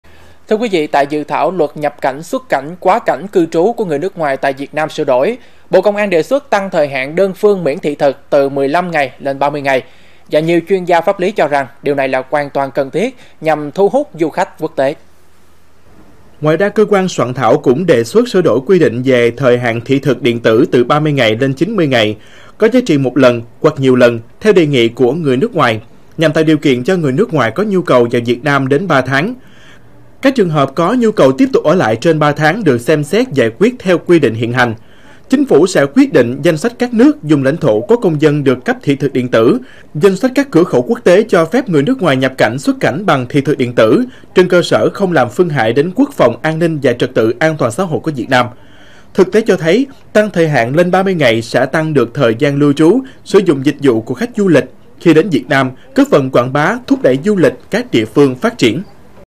Bộ Công an đề xuất tăng thời gian miễn thị thực | Cần Thơ TV
Tại Dự thảo Luật Nhập cảnh, xuất cảnh, quá cảnh, cư trú của người nước ngoài tại Việt Nam sửa đổi, Bộ Công an đề xuất tăng thời hạn đơn phương miễn thị thực từ 15 ngày lên 30 ngày, nhiều chuyên gia pháp lý cho rằng điều này là hoàn toàn cần thiết, nhằm thu hút khách du lịch quốc tế.